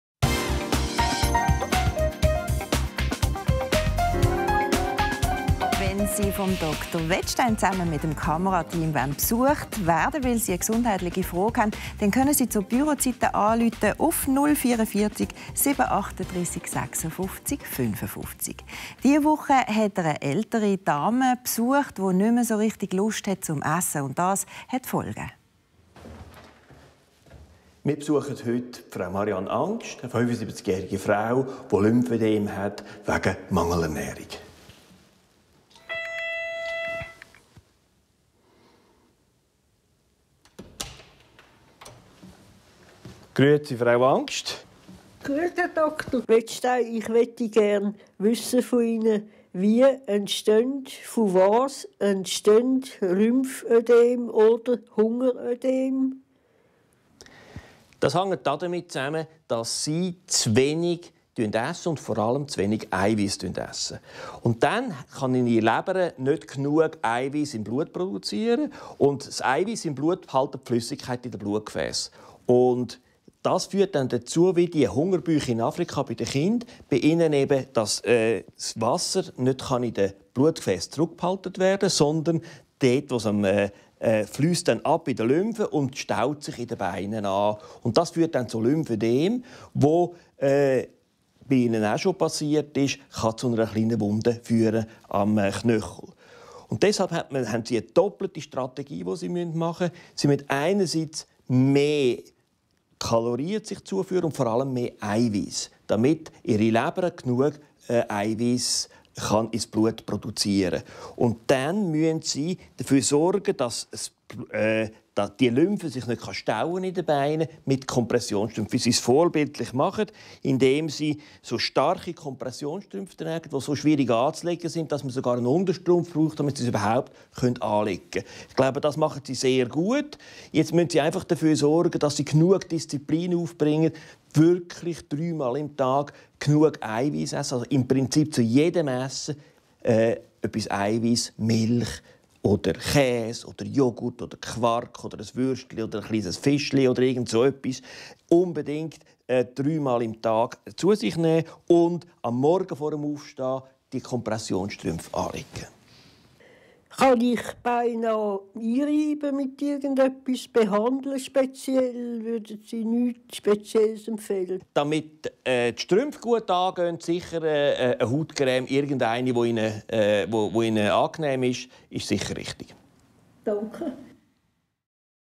Hungerödem
Marianne Angst aus Zürich isst im Alter zu wenig. Sie hatte deshalb schon ein Hungerödem - eine Wasseransammlung im Gewebe. Dr. Albert Wettstein sagt, was sie genau essen muss, damit die Wasseransammlung zurückgeht.